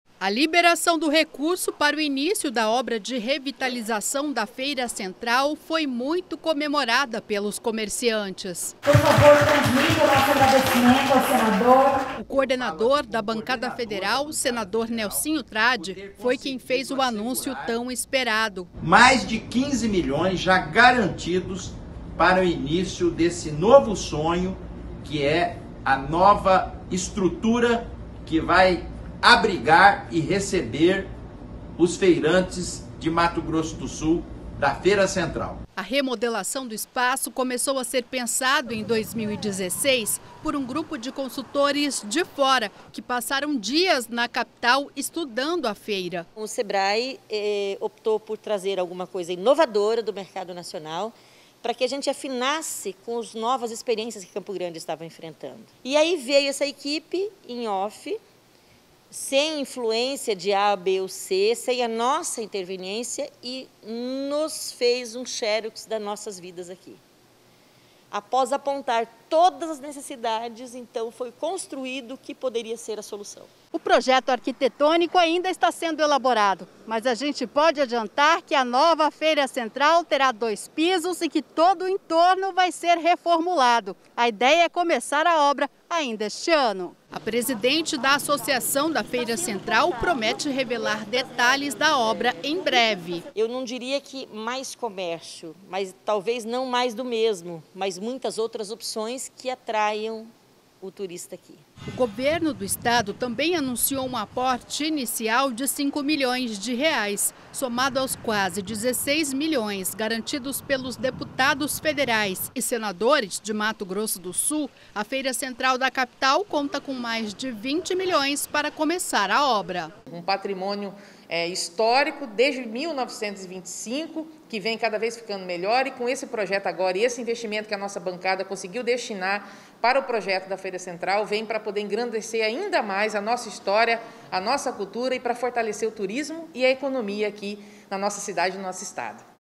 Feira central de Campo Grande terá R$ 20 milhões para início das obras de revitalização
A Feira central de Campo Grande vai passar por uma grande remodelação, e as obras estão previstas para começar ainda este ano. O investimento será de 20 milhões de reais, e foi garantido pelo poder público.